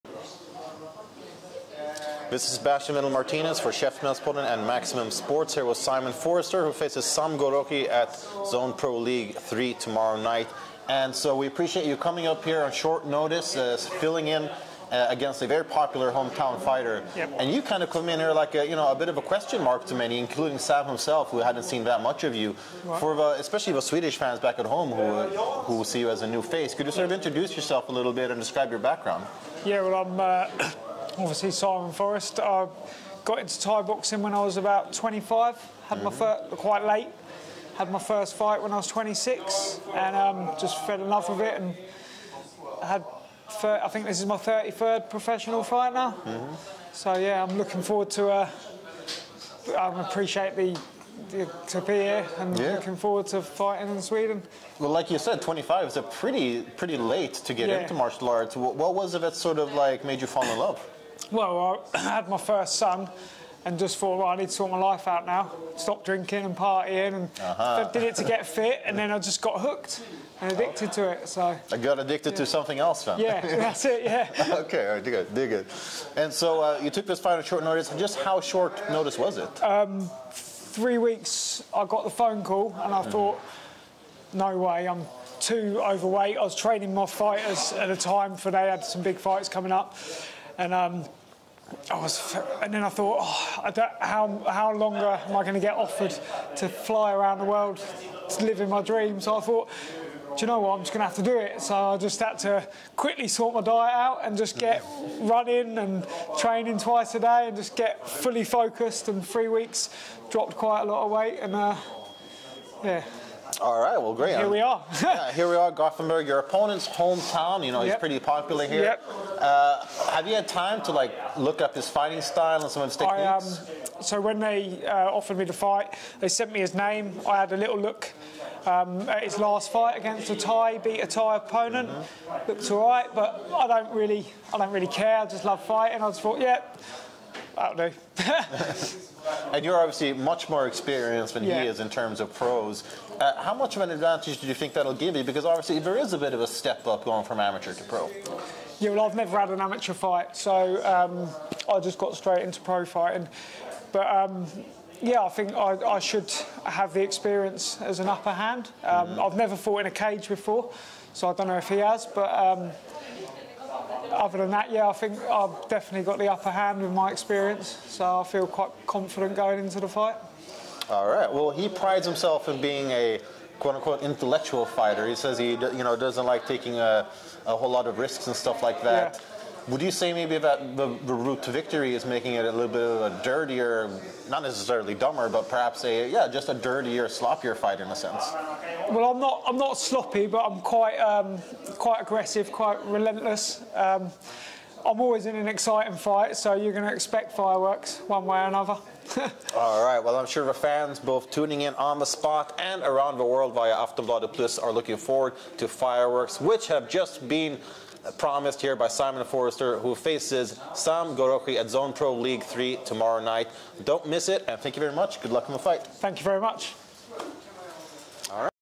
Simon Forrest tells Swedish fans to "Expect Fireworks" at Zone Pro League 3
British thaiboxer Simon Forrest stepped up on short notice to take on hometown fighter Sam Ghoroghi at Zone Pro League 3 in Gothenburg, Sweden. In his interview with Maximum Sports he discusses his decision to take the fight, his background and more!